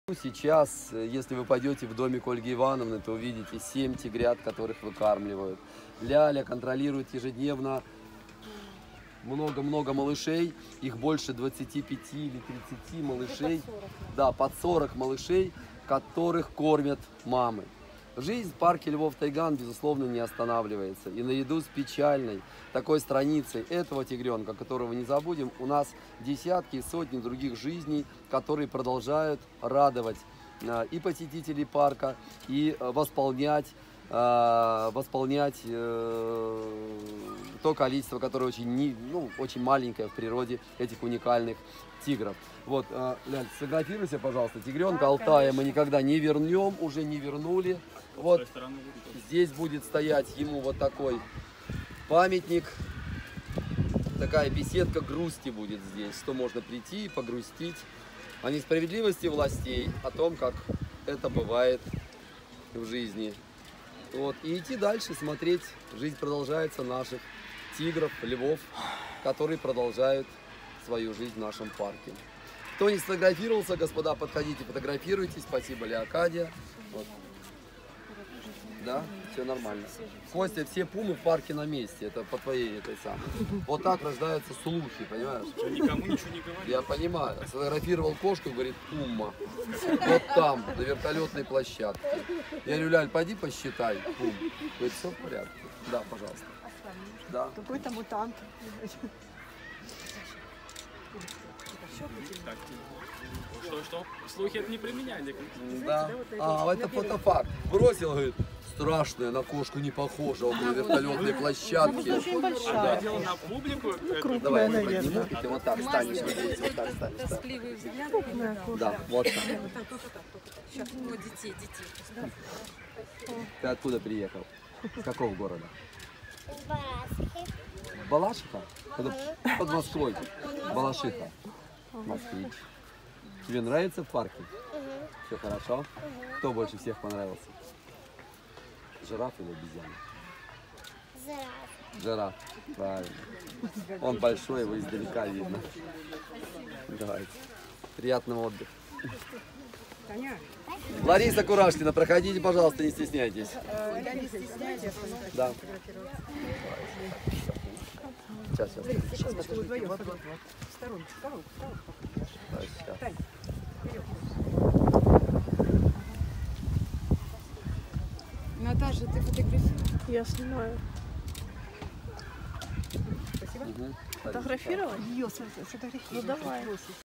Фотографирование возле памятника тигренку Алтаю
Автор: Наталья Флорес
Бронзовый Памятник тигренку Алтаю, установленный в черной беседке, в конце главной аллеи парка Тайган!
Алтай мы тебя никогда не забудем, скорбим и любим!
Фотографирование после открытия памятника тигренку Алтаю!